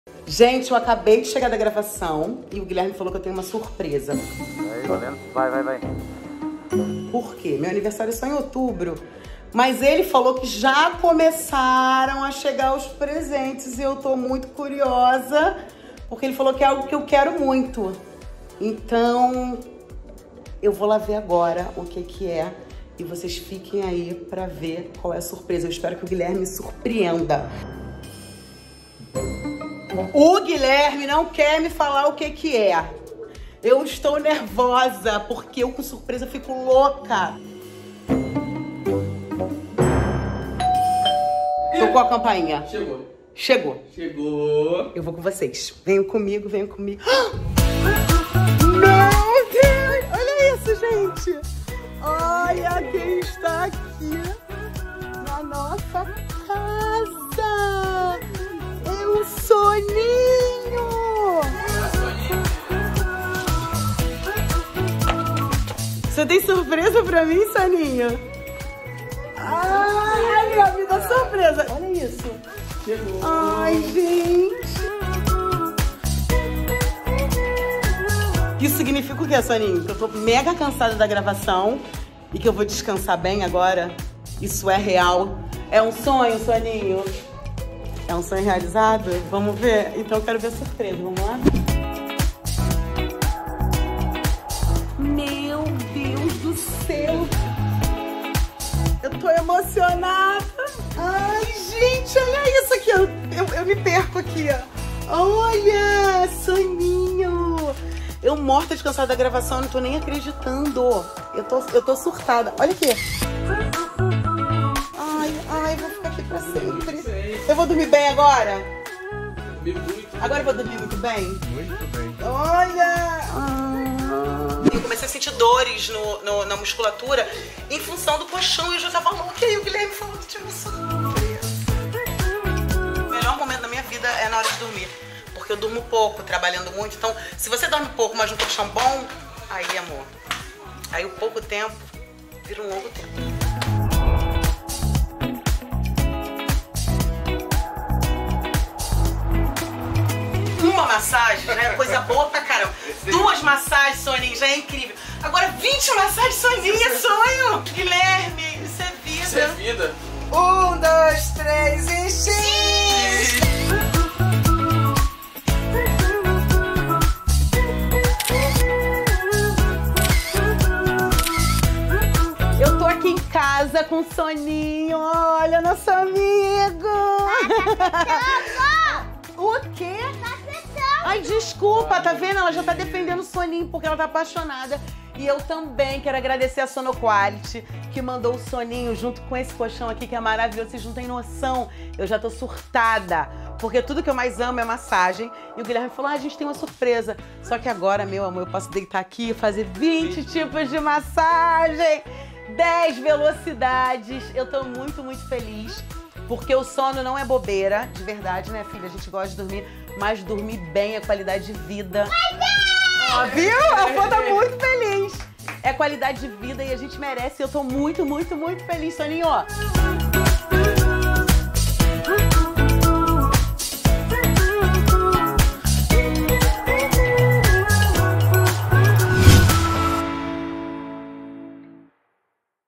A atriz Roberta Rodrigues é presenteada com o colchão tecnológico Sono Quality.
A atriz global, Roberta Rodrigues, junto com a sua família e todo seu astral, recebe em sua casa a equipe Sono Quality.

Central de Vendas: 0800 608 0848